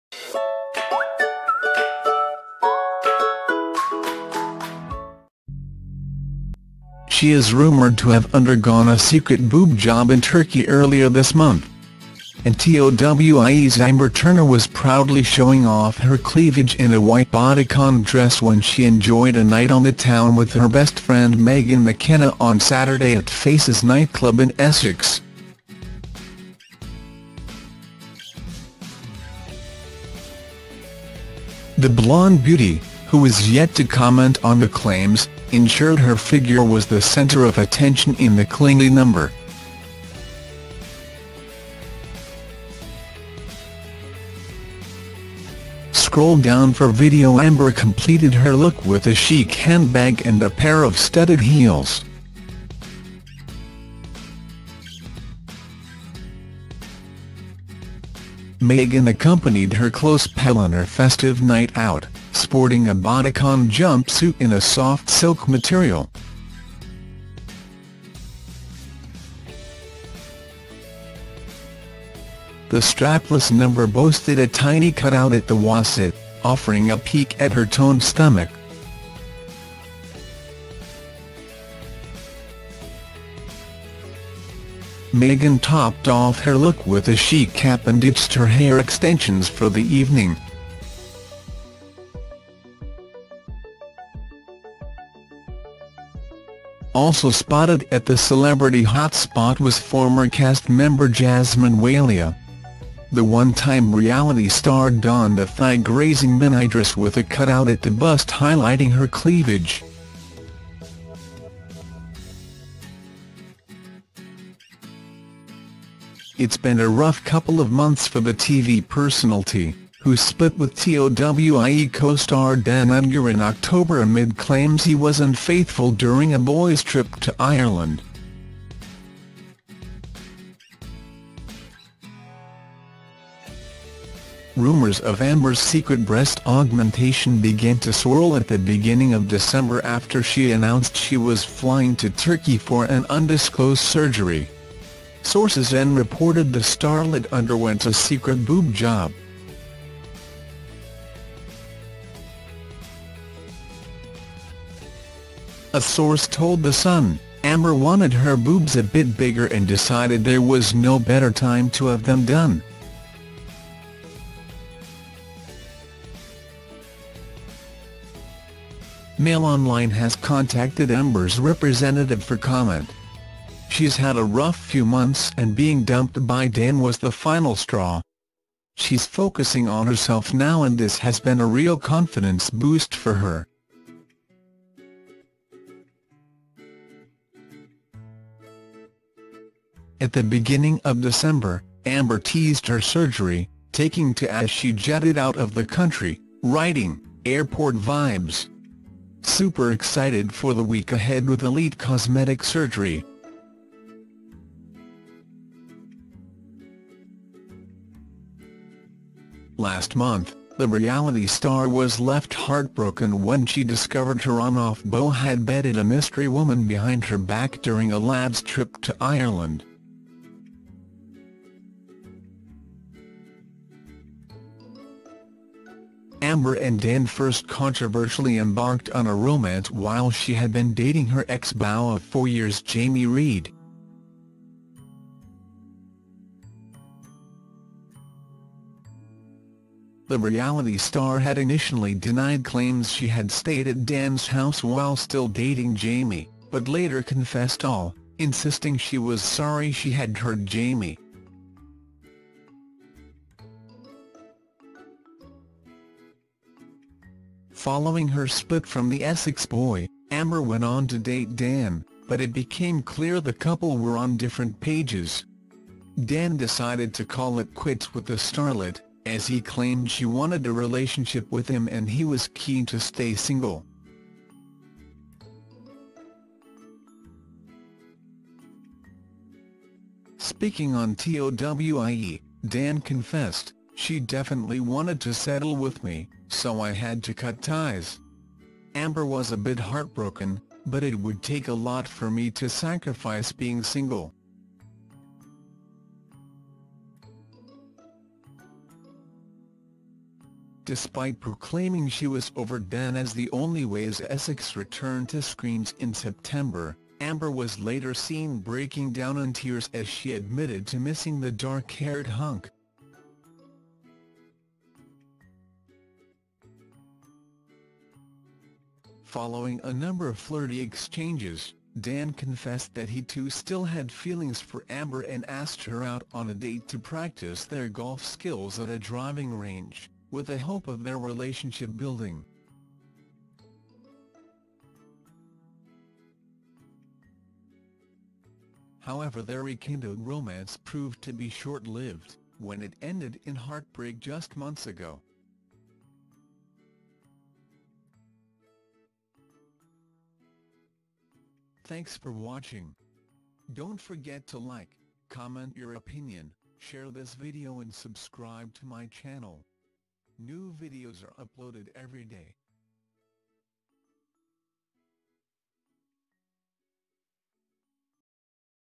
TOWIEs Amber Turner shows off boob job in bodycon dress with Megan Breaking News
She is rumoured to have undergone a secret boob job in Turkey earlier this month. And TOWIEs Amber Turner was proudly showing off her cleavage in a white bodycon dress when she enjoyed a night on the town with her best friend Megan McKenna on Saturday at faces Nightclub in EsBreaking News.